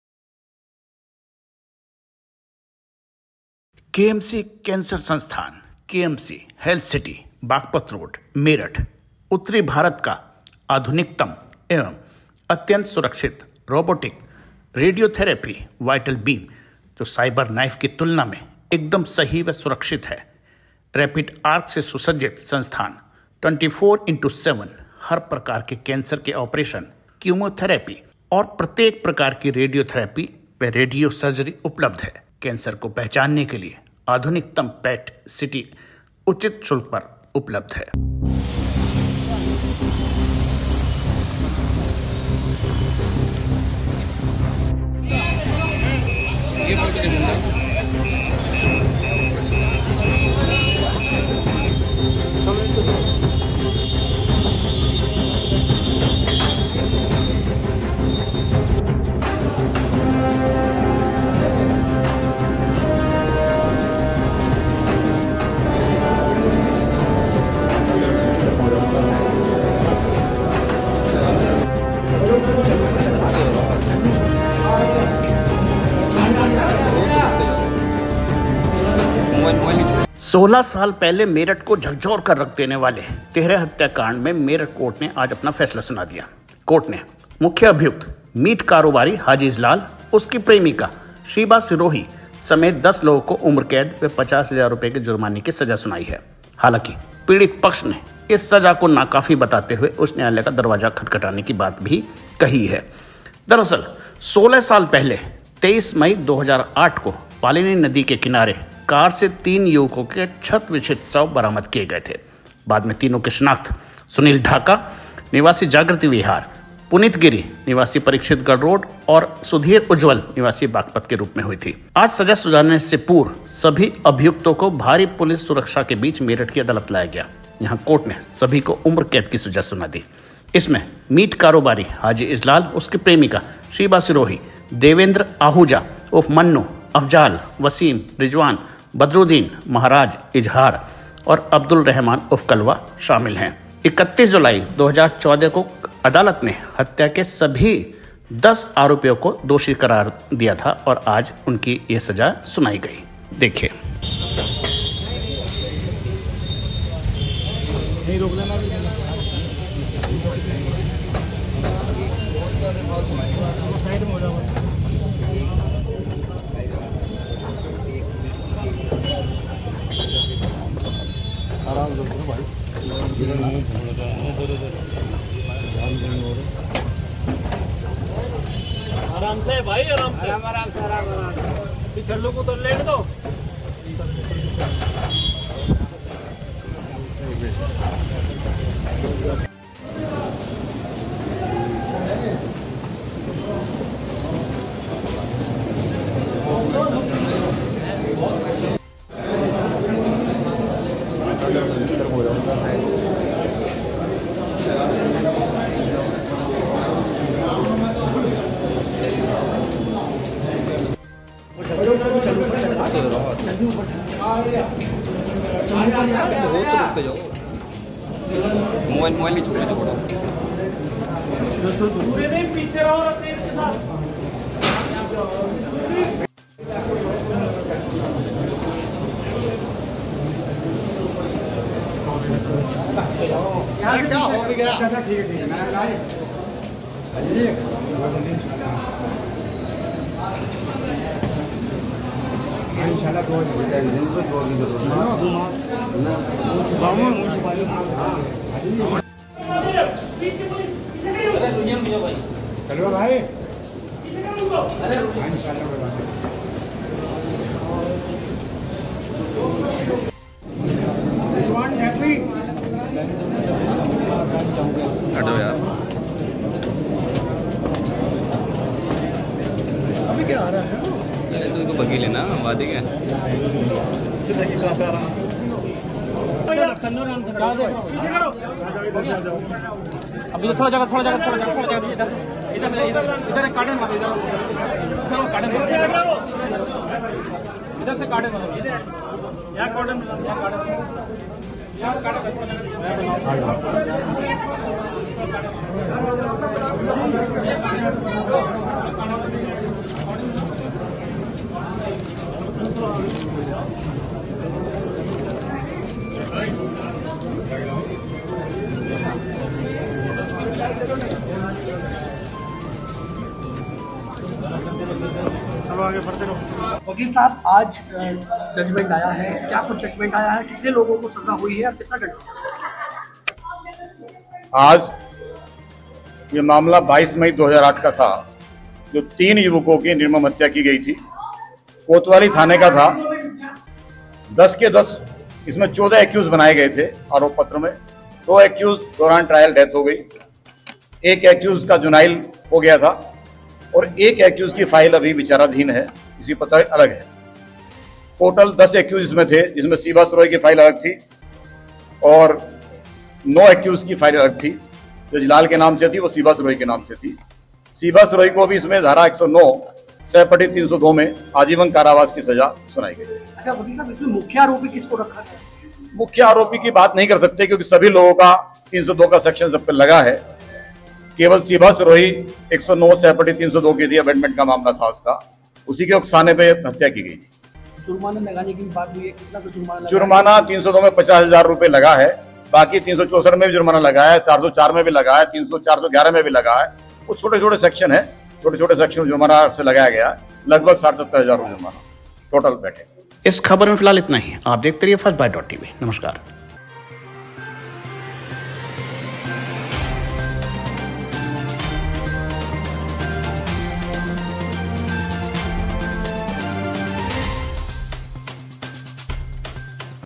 तिहरे हत्याकांड को अंजाम देने वाले मीट कारोबारी हाजी इजलाल व महबूबा को उम्रकैद First Byte.tv
गुदड़ी तिहरा हत्याकांड,
मीट कारोबारी हाजी इजलाल,
शीबा सिरोही,
लह साल पूर्व हुआ था गुदड़ी में तिहरा हत्याकांड,
हाजी इजलाल ने अपनी प्रेमिका शीबा के लिये किये थे कत्ल,
तीनों के शव कार से बैलानी नदी के पास फेंक दिये थे,
#31 जुलाई को कोर्ट ने सभी को दोषी दिया था करार,
सभी को उम्रकैद व 50 हजार के जुर्माने की सजा सुनाई,
पीड़ित परिवार ने सजा को नाकाफी है बताया ,
मीट कारोबारी इजलाल ने बचने को पैसा पानी की तरह बहाया,
हाजी शाहिद अखलाक

1.वाकई मोदीजी की तुलना नेहरूजी से नहीं की जा सकती